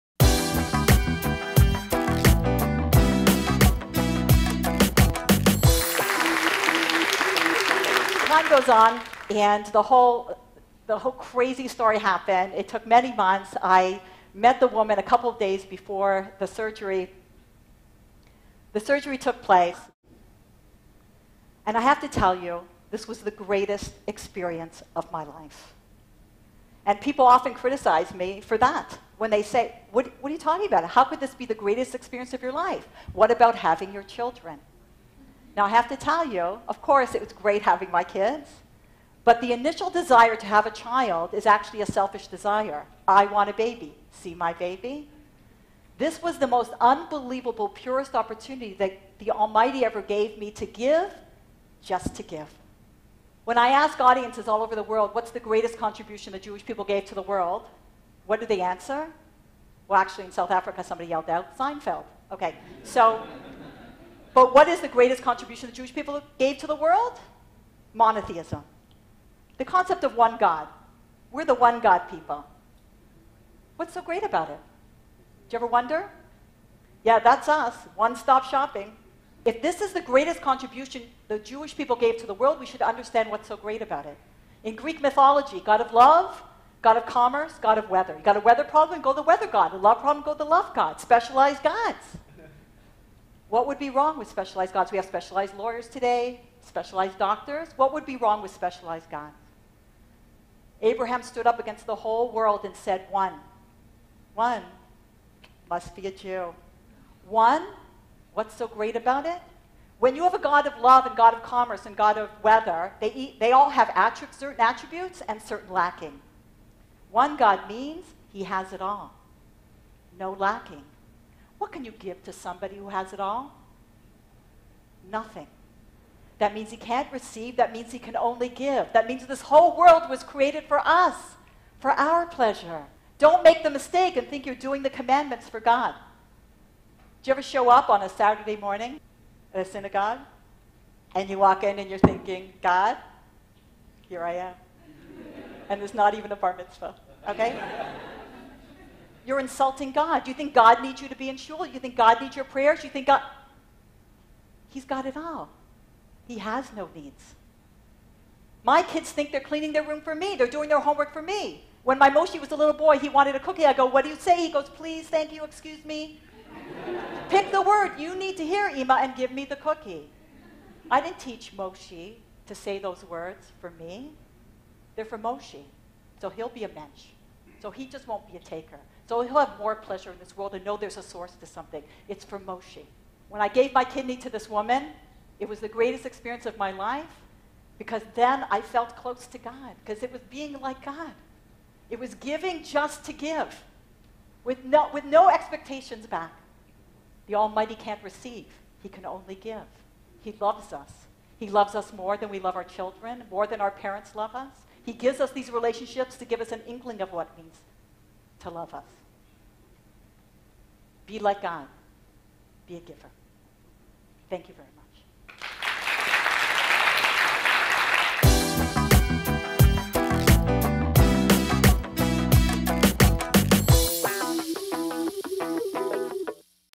Lori's "Greatest Experience in My Life"
Lori Palatnik shares her greatest experience of her life and there by explains reason of creation.